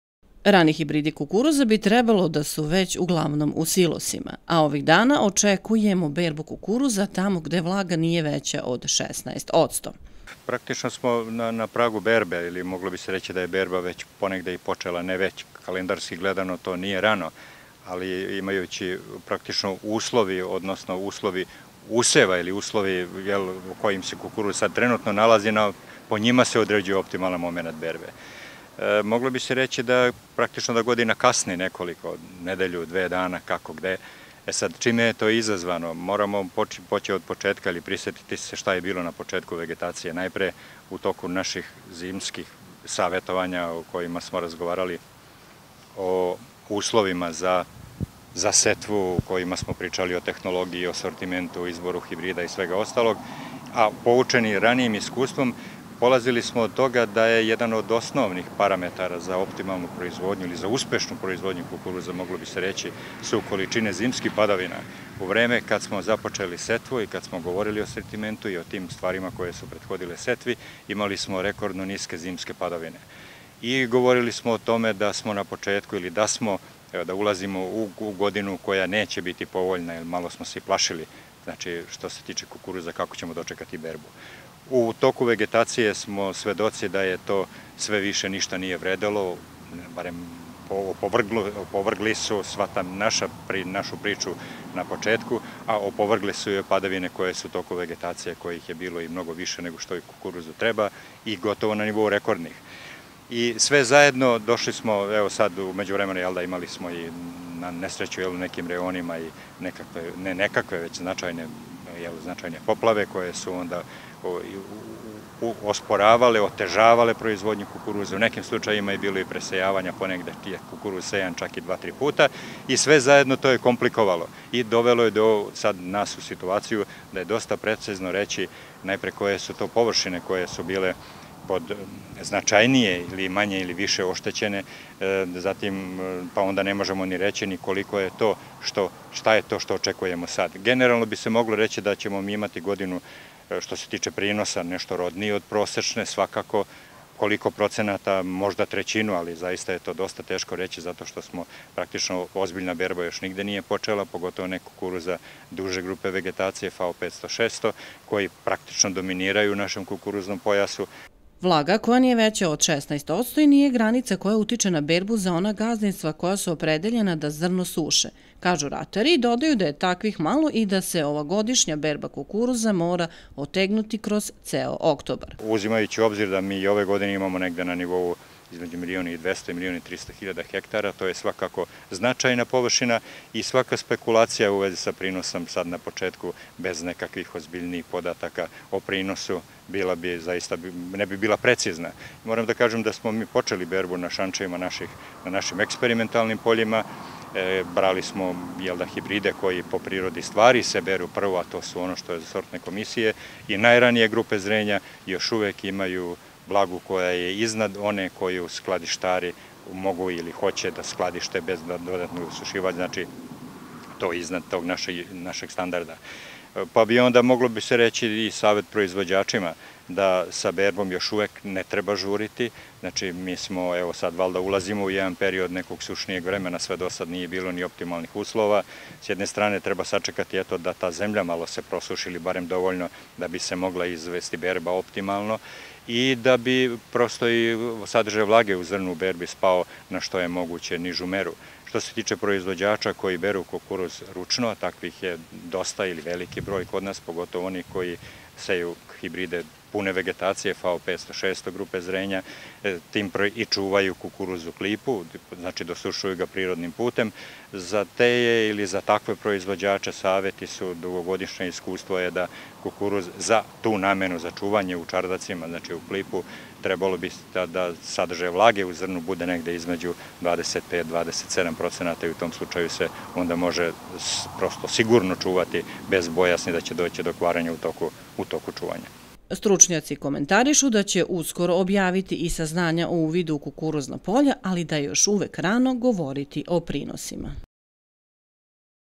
Dr Milisav Stojaković o berbi kukuruza 2014. godine
Video prilog sa savetima dr Milisava Stojakovića za žetvu kukuruza 2014. godine možete da pogledate i na sajtu Instituta za ratarstvo i povrtarstvo kao i na zvaničnoj Facebook strani Instituta